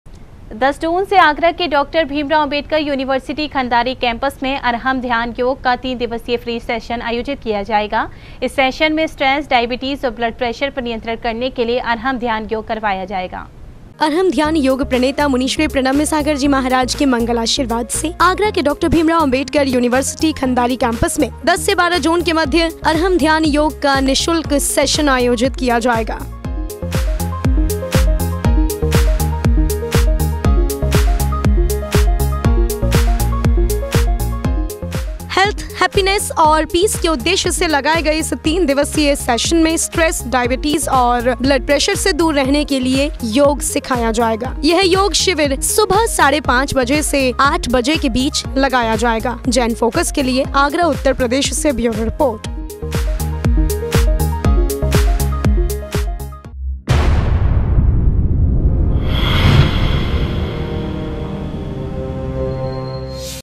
Agra News:आगरा यूनिवर्सिटी के खंदारी कैंपस में होगा अर्हं ध्यान योग | JAIN FOCUS
आगरा यूनिवर्सिटी के खंदारी कैंपस में होगा अर्हं ध्यान योग
10 से 12 जून के मध्य लगेगा तीन दिवसीय शिविर
सुबह 5:30 से 8 बजे तक करवाया जायेगा योग